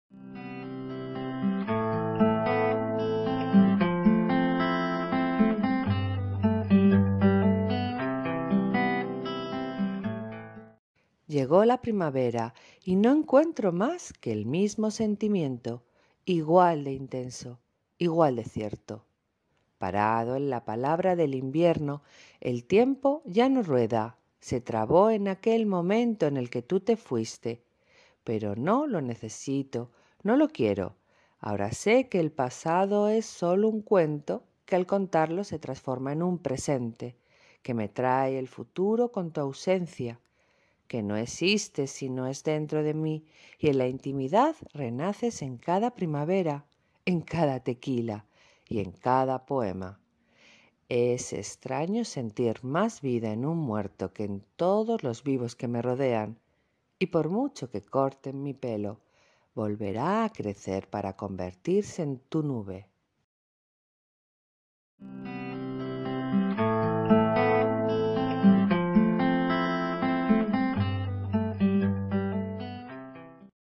Cuento
Llegó la primavera
y no encuentro más que el mismo sentimiento,
igual de intenso,
igual de cierto.
Parado en la palabra del invierno,
el tiempo ya no rueda,
se trabó en aquel momento
en el que tú te fuiste.
Pero no lo necesito,
no lo quiero.
Ahora sé que el pasado es solo un cuento,
que al contarlo se transforma en un presente
que me trae el futuro con tu ausencia
que no existes si no es dentro de mí
y en la intimidad renaces en cada primavera,
en cada tequila y en cada poema.
Es extraño sentir más vida en un muerto
que en todos los vivos que me rodean
y por mucho que corten mi pelo
volverá a crecer para convertirse en tu nube.